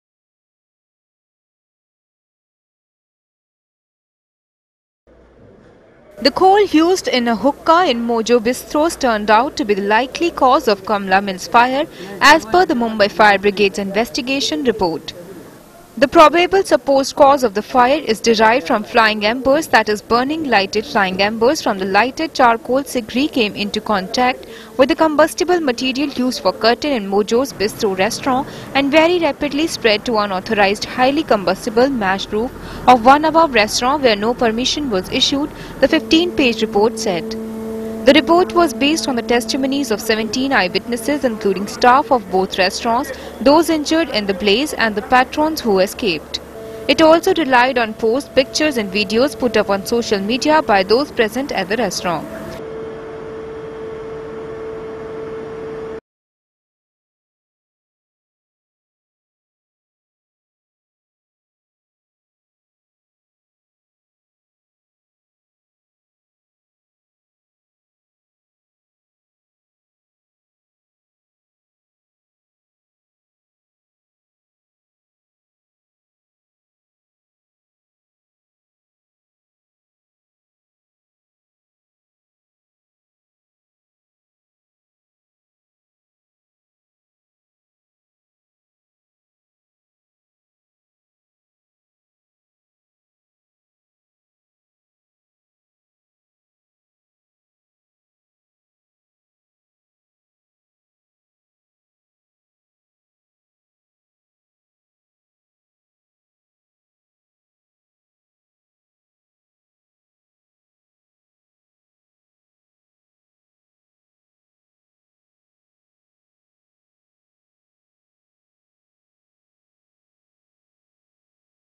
Hookah embers caused Kamala Mills fire: Fire dept
The coal used in a hookah in Mojo’s Bistro turned out to be the likely cause of Kamala Mills fire, as per the Mumbai Fire Brigade’s investigation report. "The probable supposed cause of the fire is derived from 'Flying Embers' i.e Burning/lighted flying embers from the lighted charcoal segree came into contact with the combustible material used for curtain in Mojo's Bistro Restaurant and very rapidly spread to unauthorised highly combustible thatched roof of 1 Above Restaurant (where no permission was issued)," the 15-page report said. The report was based on the testimonies of 17 eyewitnesses including staff of both restaurants, those injured in the blaze, and the patrons who escaped. It also relied on posts, pictures and videos put up on social media by those present at the restaurant.